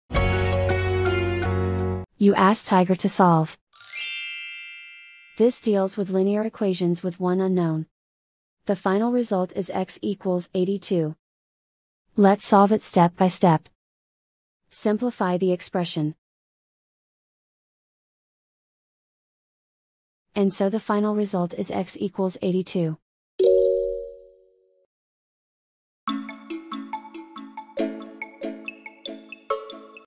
Linear equation with one unknown: Solve x=180-74-24 step-by-step solution
Solving x=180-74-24 using the linear equation with one unknown solver

for a step-by-step and more tips and tricks for dealing with linear equations with one unknown.